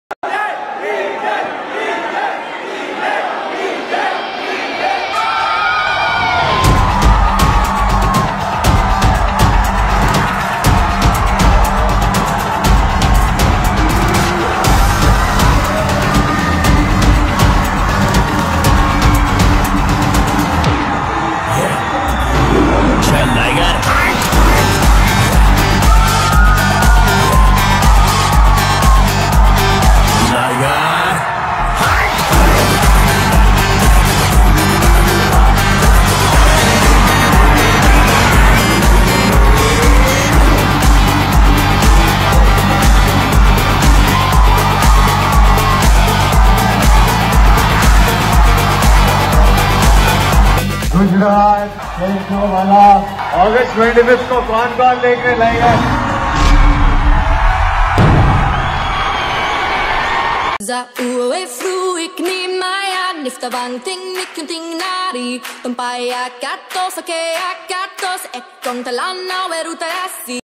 Vijay devarakonda Tour at Ahmedabad is all about your over the roof Energy💥LIGER
Thank you for your support to "Gemini Bharat News " channel🙏